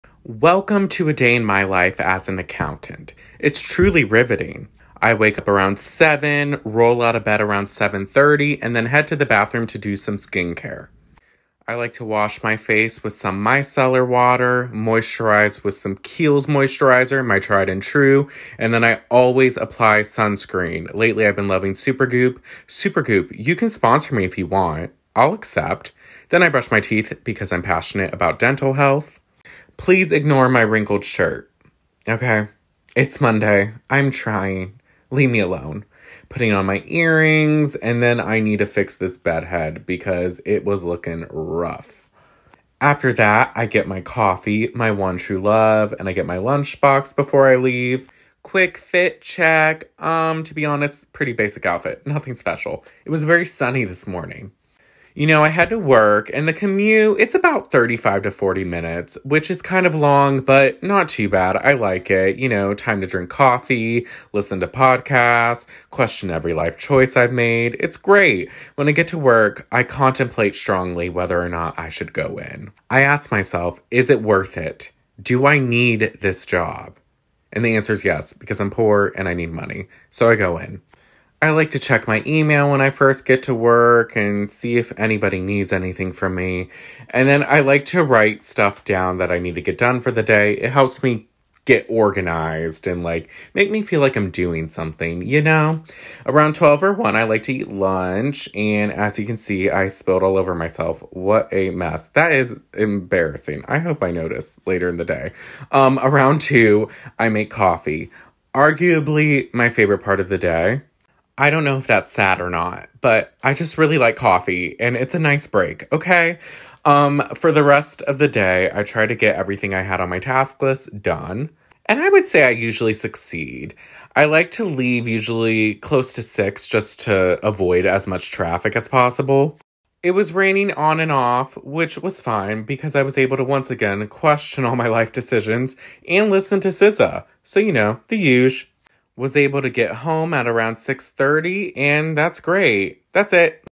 A Day In My Life As An Accountant
Welcome to a typical workday in my life! Hope you enjoy :)